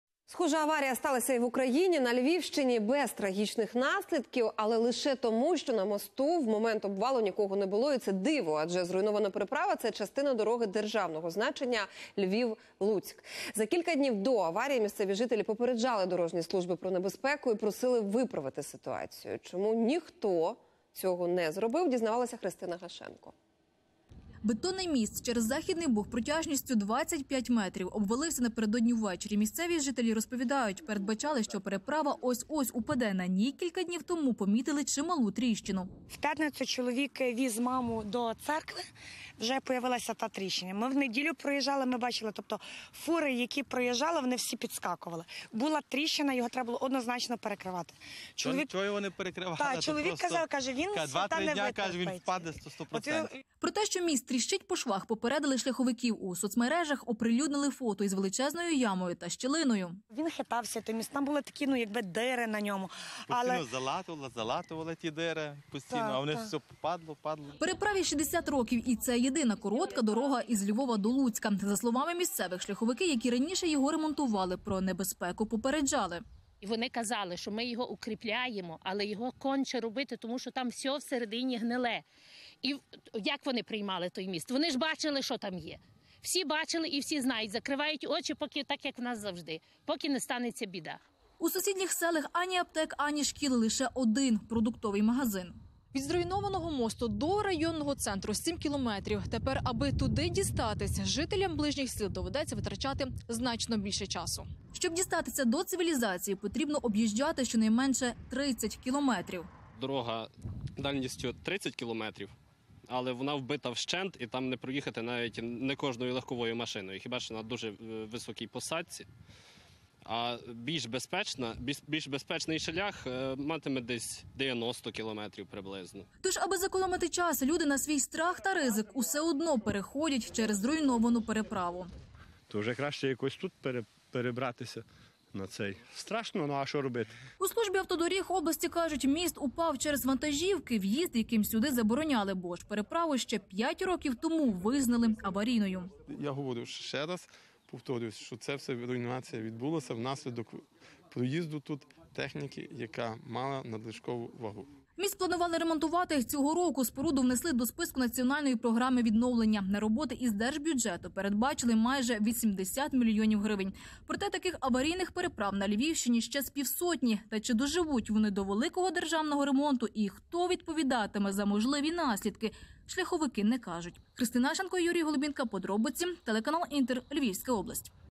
На Львівщині обвалився міст державного значення
За кілька днів до аварії місцеві жителі керівників дорожніх служб попереджали про можливу небезпеку на переправі. Люди помітили новоутворену тріщину то ж просили виправити ситуацію. Чому, цього ніхто не зробив?